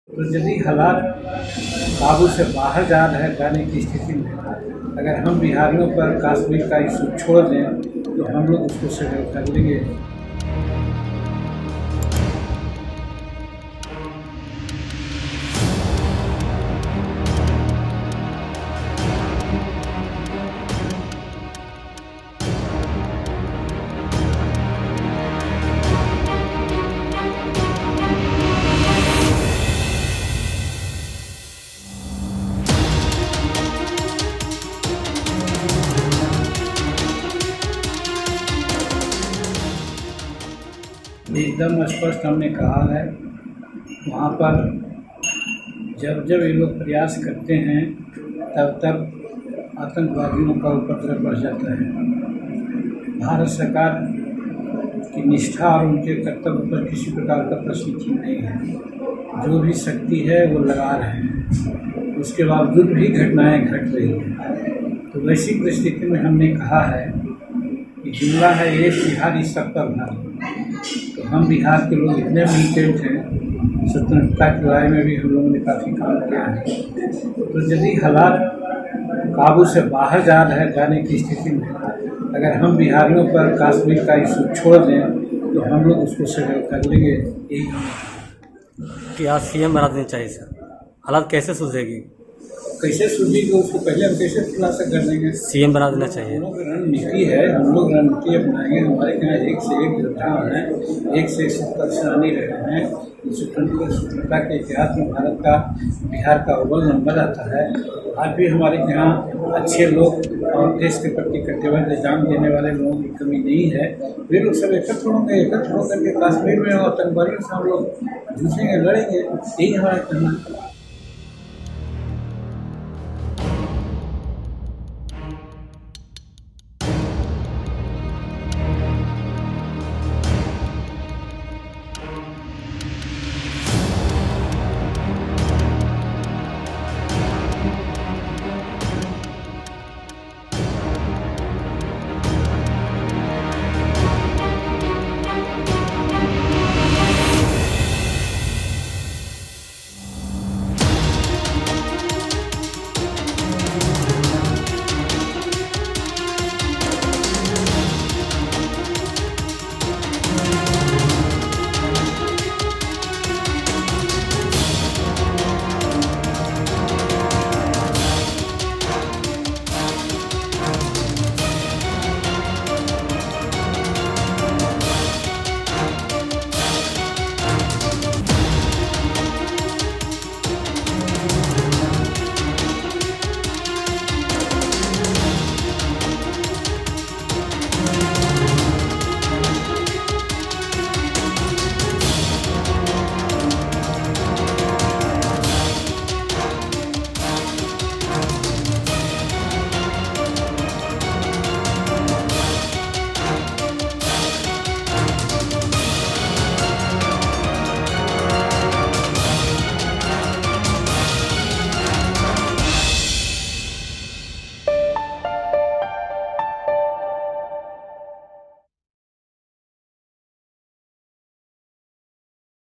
'Let Biharis handle Kashmir…': As terrorists kill migrant workers, ex-CM Manjhi appeals to PM Modi
Former Bihar CM Jitan Manjhi has a request for PM Modi, HM Shah over Kashmir killings. Manjhi urged PM Modi to leave responsibility on Biharis to improve the situation in the Valley. Manjhi said he has full faith in Modi govt but terror-related incidents are still taking place. Manjhi's remarks came amid a slew of targeted killings that has sparked fear in Kashmir. Jitan Manjhi's party - Hindustani Awam Morcha (Secular) - is an NDA partner in Bihar. Manjhi earlier expressed his anger over frequent attacks against civilians, especially Biharis. He said terror attack-related issues will be solved if J&K is handed over to Biharis for 15 days. Terrorists have stepped up attacks against minority communities, non-locals living in Kashmir. In the last few days, terrorists have killed five hawkers & labourers from Bihar, causing panic among migrants.